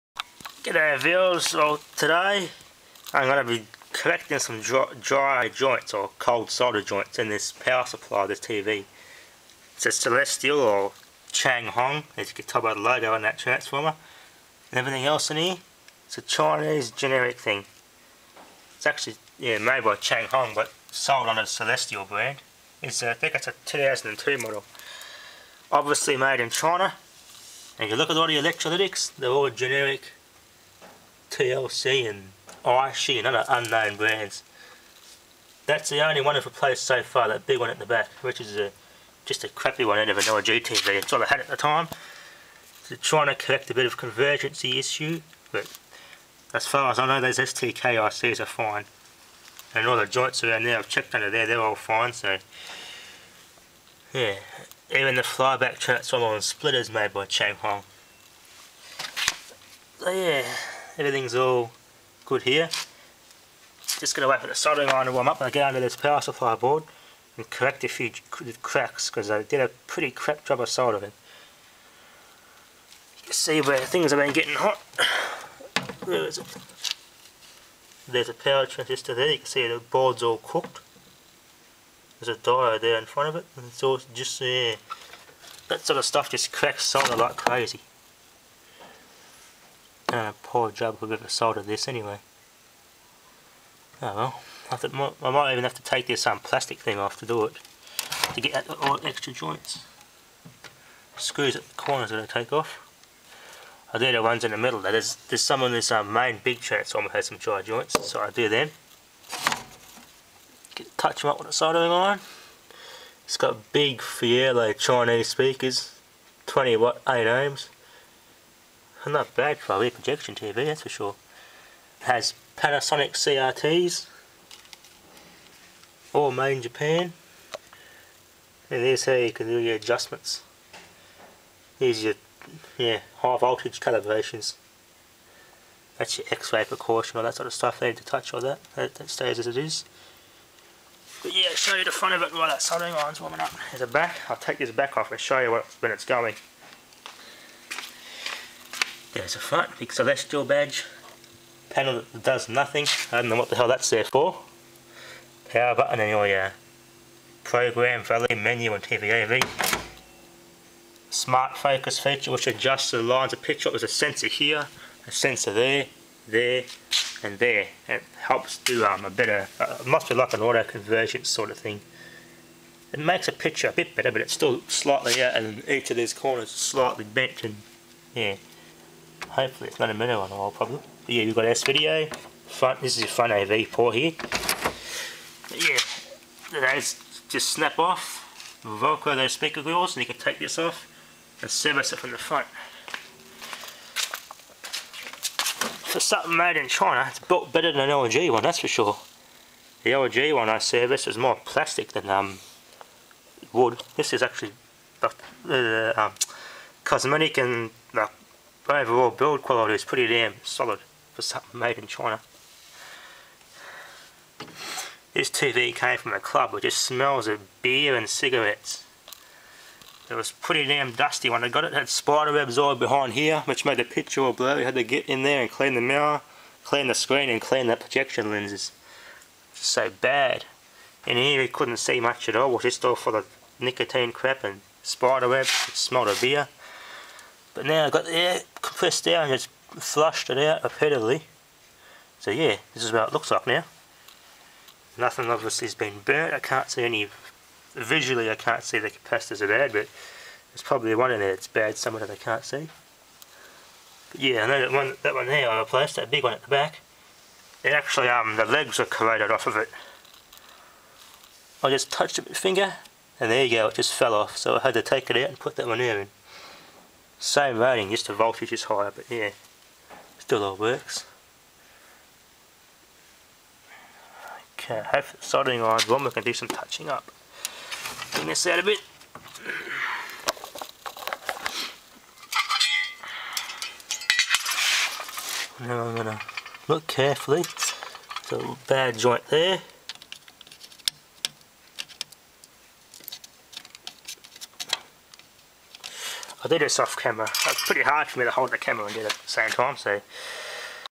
Basic repair & look inside an RP TV Part 1
A look inside a Rear Projection TV